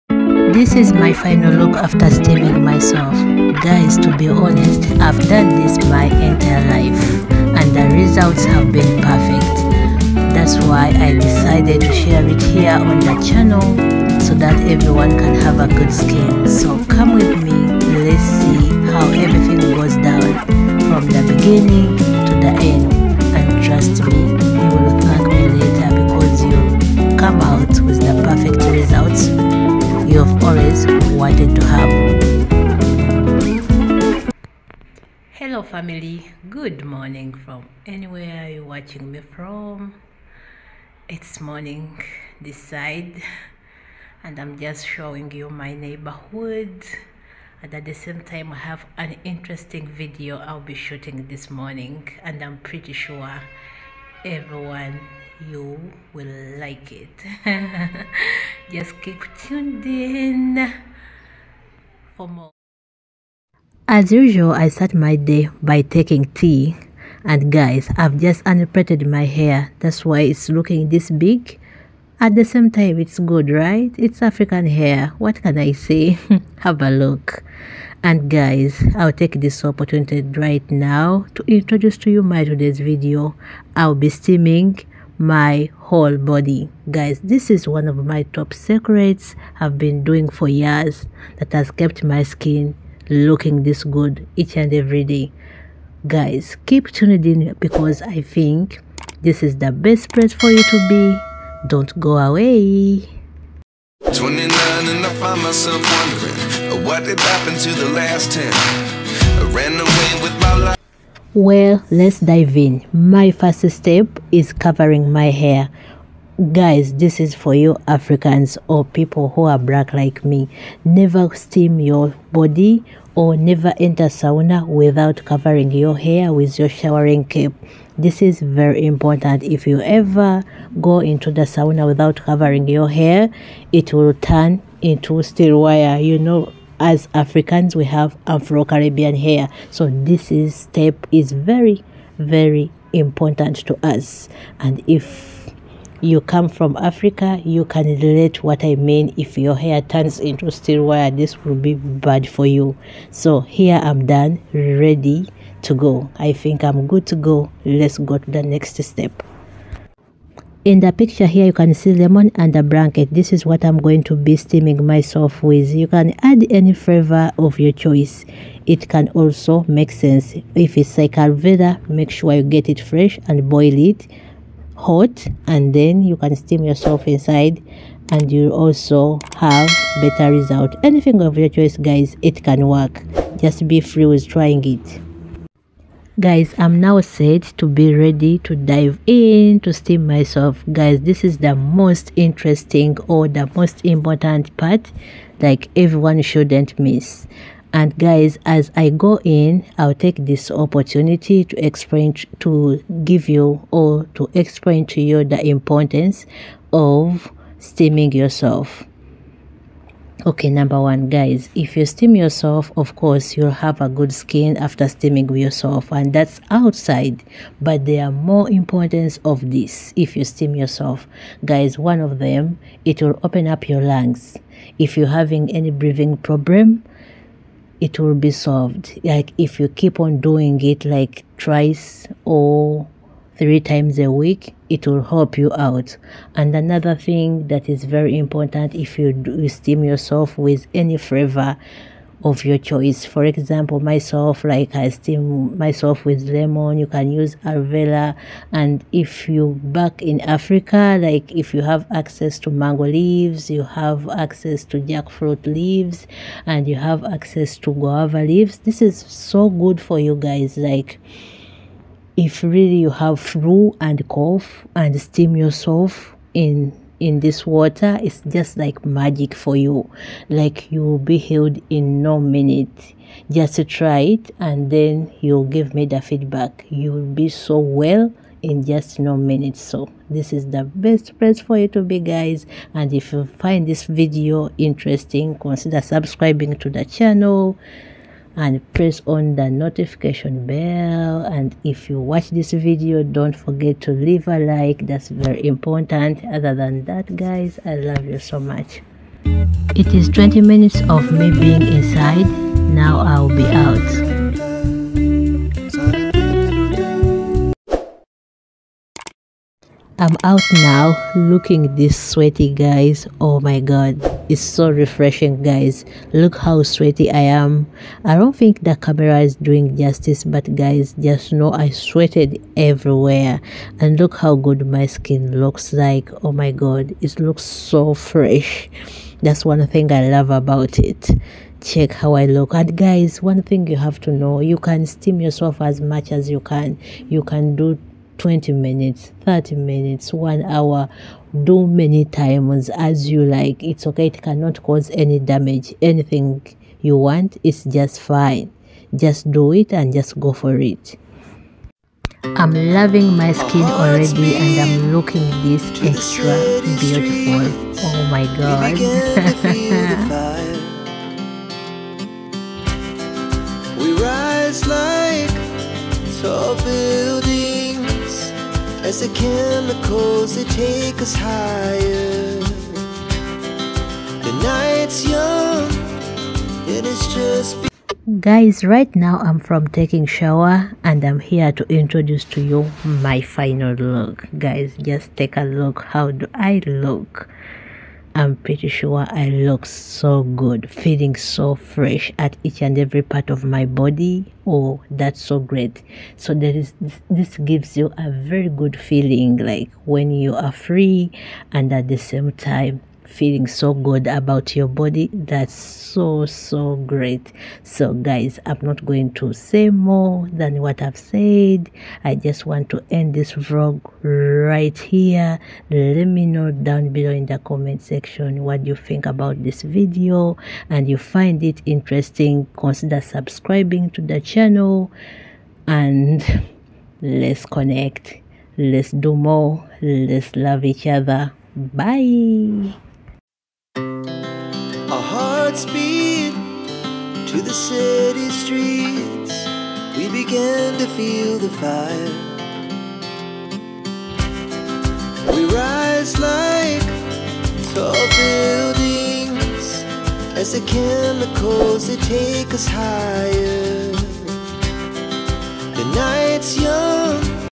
Traditional African body steaming#health
This video explains the importance of steam yr body either 2 or 3times a week

Activements
1, Best skin
2,It opens up  lungs if u have breathing issues
3, if u have 😷 flu en cough, u just feel better after the steaming.